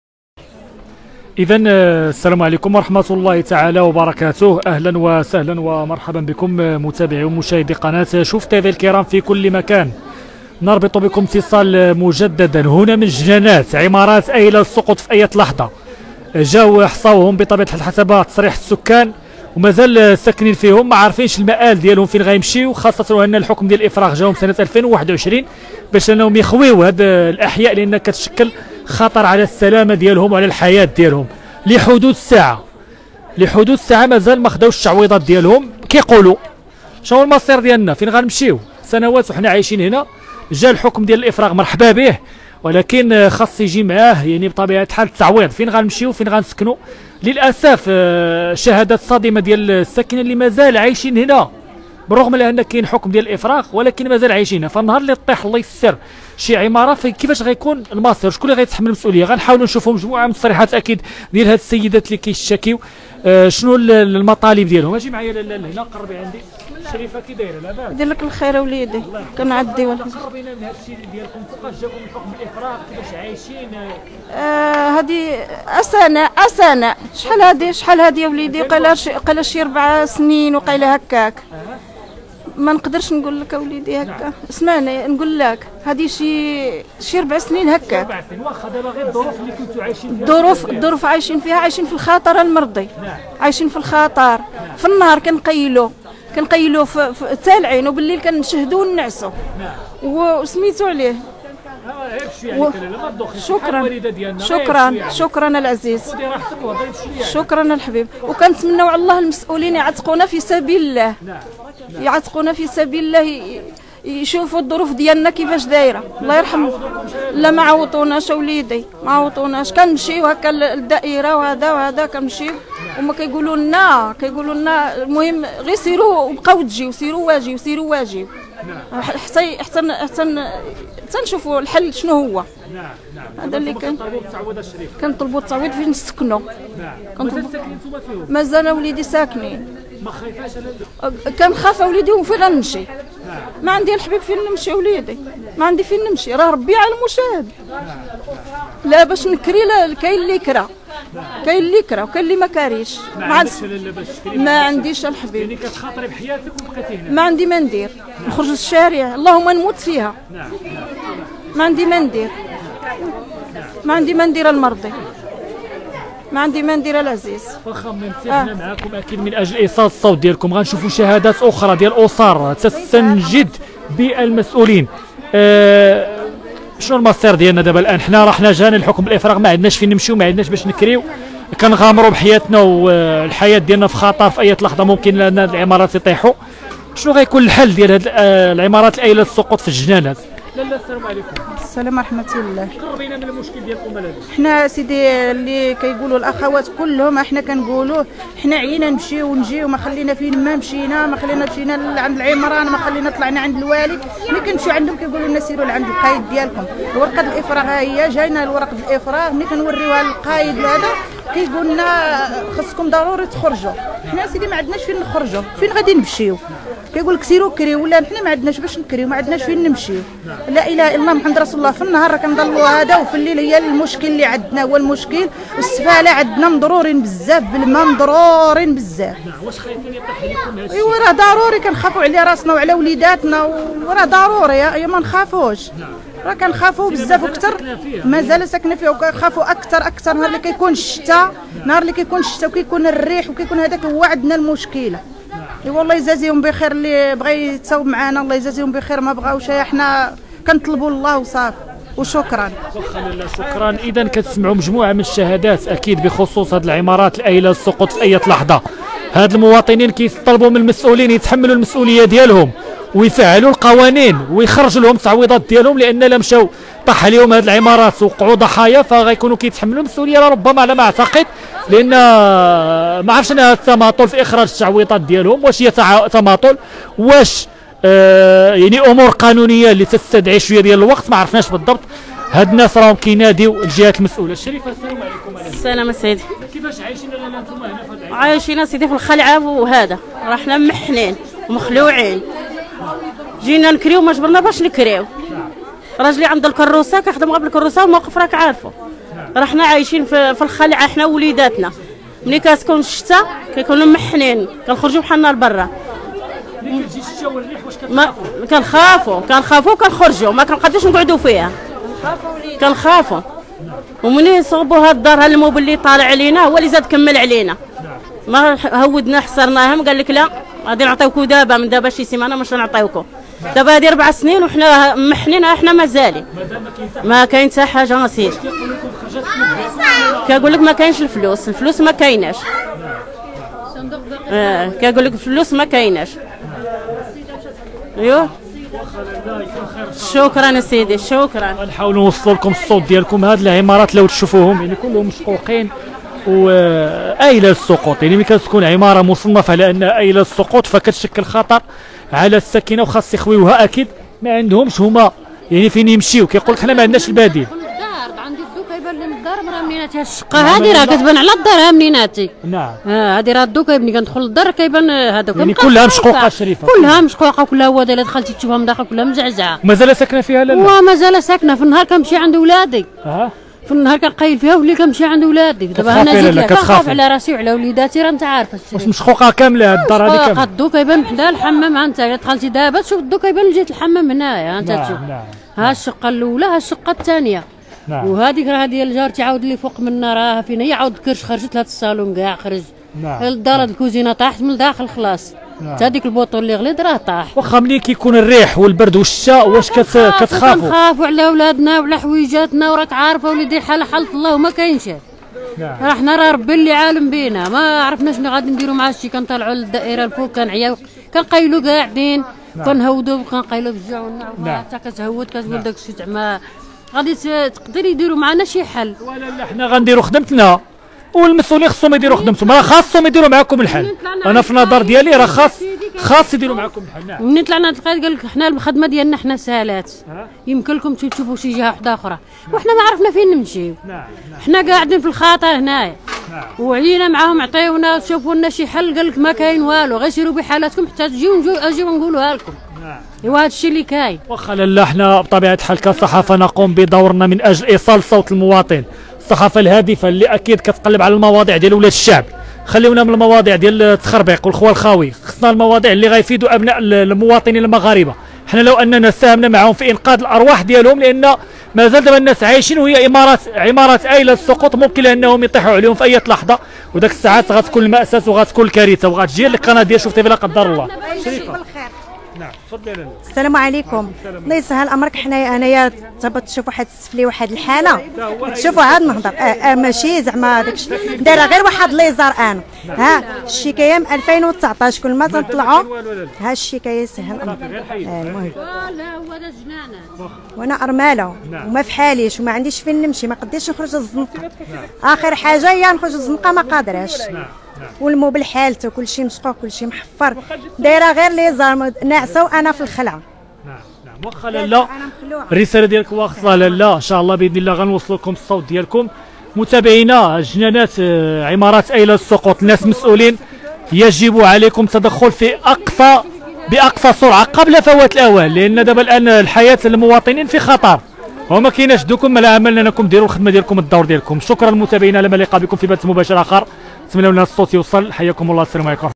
سكان الجنانات جاليهم قرار الإفراغ بسبب المنازل الآيلة للسقوط لي عايشين فيها: "بغينا حقنا فالتعويض"
عاجل ومباشرة من فاس : فين غيمشيو هاد الناس؟ سكان الجنانات جاليهم قرار الإفراغ بسبب المنازل الآيلة للسقوط لي عايشين فيها: "بغينا حقنا فالتعويض"